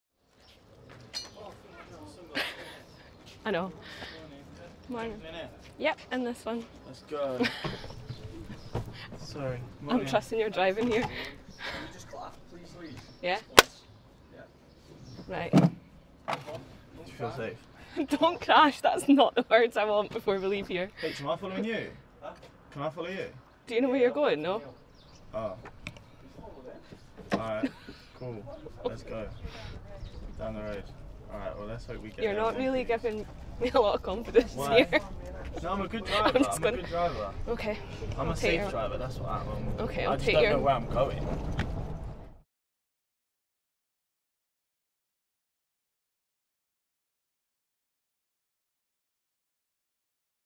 GERS IN GERMANY | The Road to Training | Trailer | 9 July 2023
Todd Cantwell gets behind the wheel with Louise Shaw as Rangers begin training in Germany.